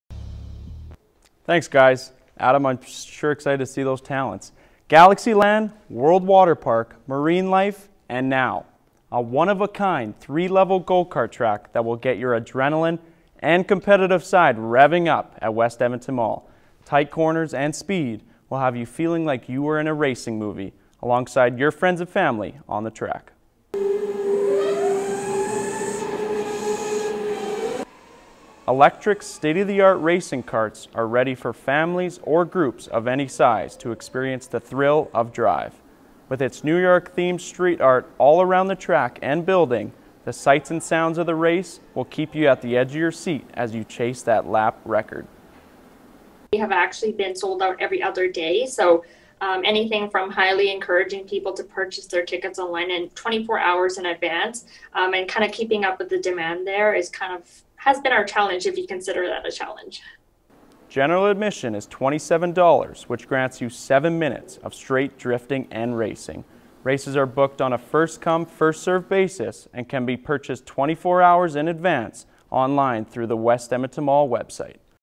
DRIVE West Edmonton Mall
West Edmonton Mall's new biggest attraction is one of North America's only three level indoor go-kart track featuring state-of-the-art electric karts.